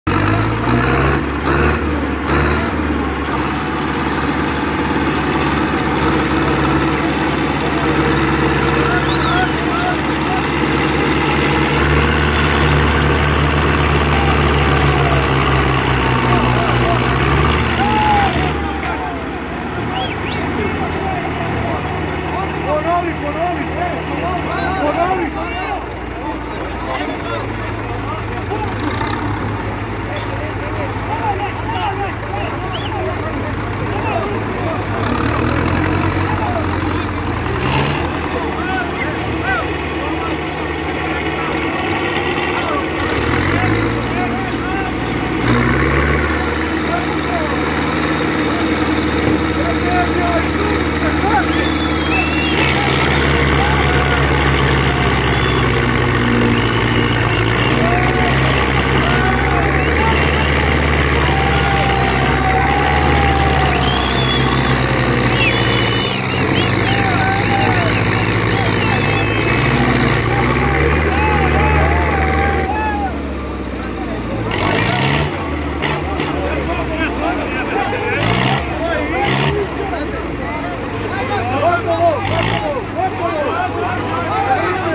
Zetor 6911 vs Zetor 5718 Najbolja borba Traktorijada Kozarska Dubica 2021
Traktorijada Kozarska Dubica 2021